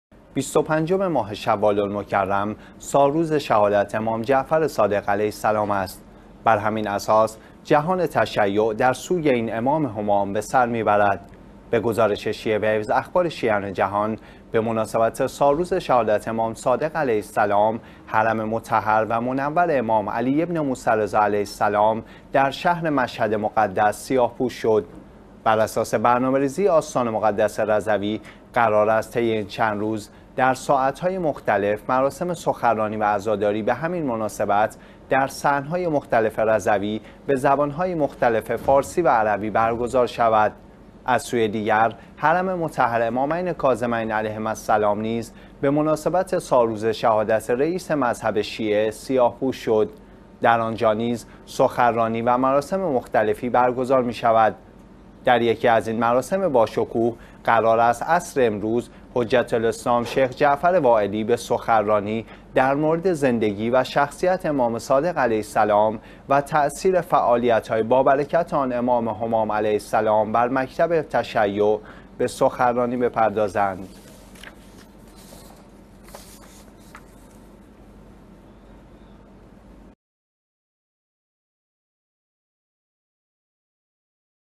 جهان تشیع در سوگ شهادت امام جعفر صادق علیه السلام
25 ماه شوال المکرم سالروز شهادت امام جعفر صادق علیه السلام است. بر همین اساس جهان تشیع در سوگ این امام همام به سر می برد.
به گزارش شیعه ویوز (اخبار شیعیان جهان)؛ به مناسبت سالروز شهادت امام صادق علیه السلام، حرم مطهر و منور امام علی بن موسی الرضا علیه السلام در شهر مشهد مقدس سیاه پوش شد.
بر اساس برنامه ریزی آستان مقدس رضوی، قرار است طی این چند روز در ساعت های مختلف مراسم سخنرانی و عزاداری به همین مناسبت در صحن های مختلف رضوی به زبان های مختلف فارسی و عربی برگزار شود.
از سوی دیگر حرم مطهر امامین در کاظمین علیهما السلام نیز به مناسبت سالروز شهادت رئیس مکتب شیعه، سیاه پوش شد.
در آنجا نیز سخنرانی و مراسم مختلفی برگزار می شود. در یکی از این مراسم با شکوه، قرار است عصر امروز حجت الاسلام شیخ "جعفر وائلی" به سخنرانی در مورد زندگی و شخصیت امام صادق علیه السلام و تأثیر فعالیت های با برکت آن امام همام علیه السلام بر مکتب تشیع، به سخنرانی بپردازند.